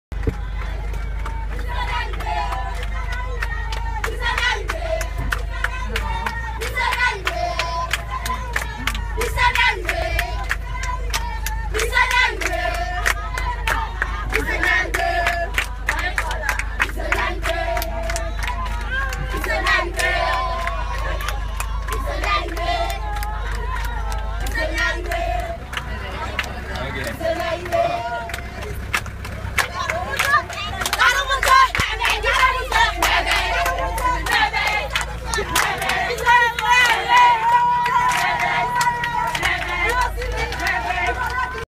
Our Welcome into the Village of Balalay
This video was recorded by Diane Hoover, a team member who came down last week with a beautiful team with hearts for serving and loving others! Each day the team went out to different villages and were able to visit and distribute clothes and food! I had the privilege to go along as a translator! One day we drove back to this village three times in one day and every time we were greeted with the same welcome song and dance! I was so excited that Diane captured this on her phone!